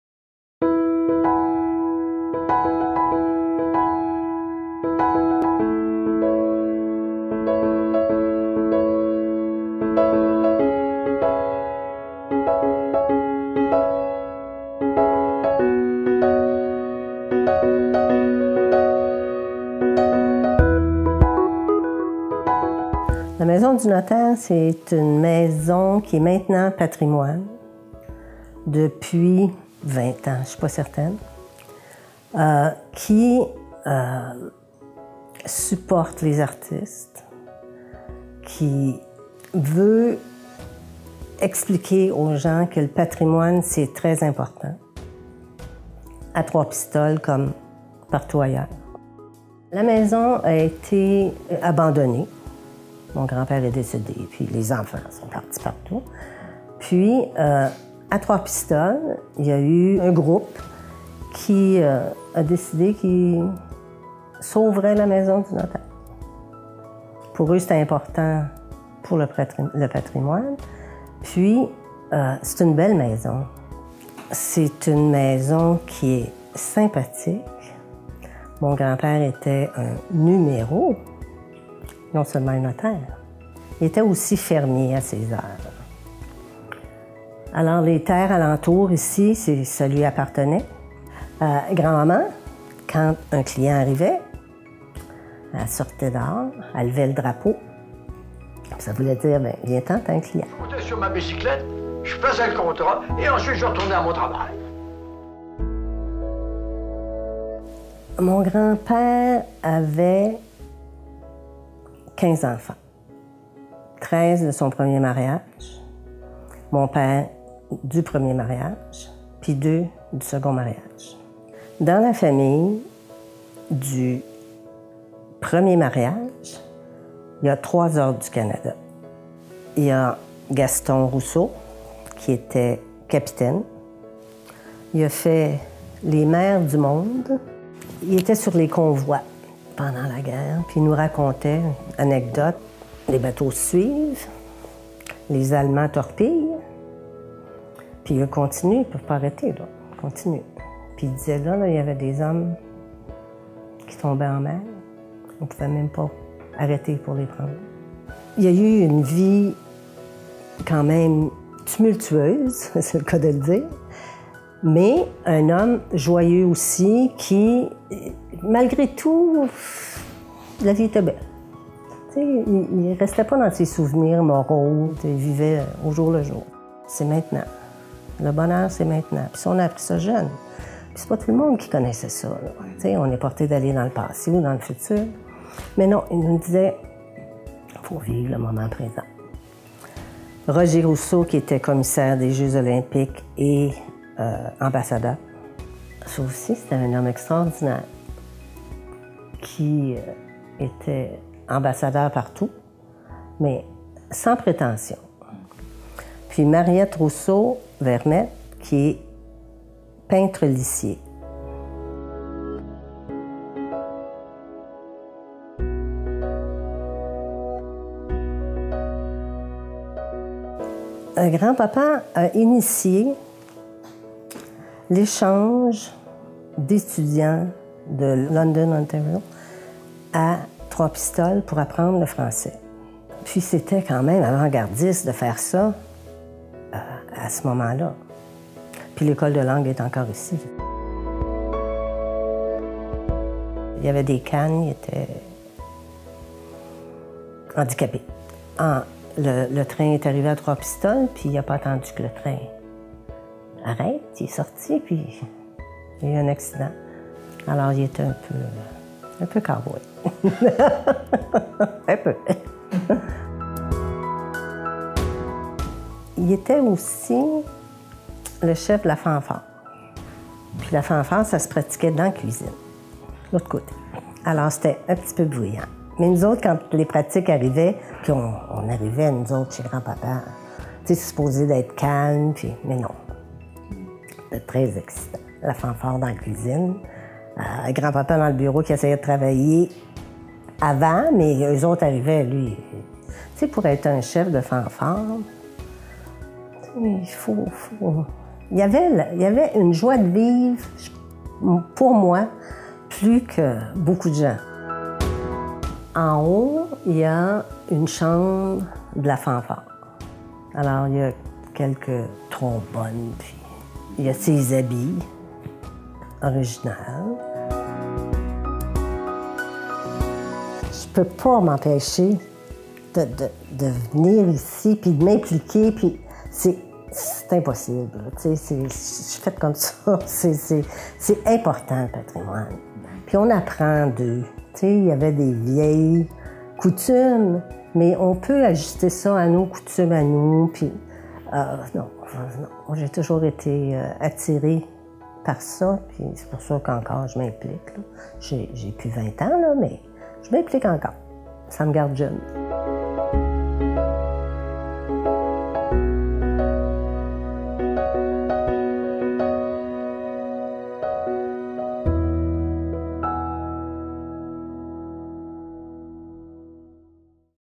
La MAISON DU NOTAIRE de Trois-Pistoles
Le mercredi 9 juin 2021, notre reporter a rencontré Mme Lyse Rousseau, présidente du CA de La Maison du Notaire de Trois-Pistoles, mais également la petite-fille du fameux notaire Rousseau, pour qu'elle explique en quoi consistent les activités de La Maison du Notaire.

Tantôt galerie d'art, tantôt élément du patrimoine pistolois, cette maison recèle d'anecdotes et de trésors patrimoniaux. Mme Rousseau nous en révèle quelques-uns, dont les habits de la fanfare du notaire Rousseau qui en était le chef.

Réalisation : TCBH
Invitée : Lyse Rousseau
Caméra et Montage : Mark-André Pépin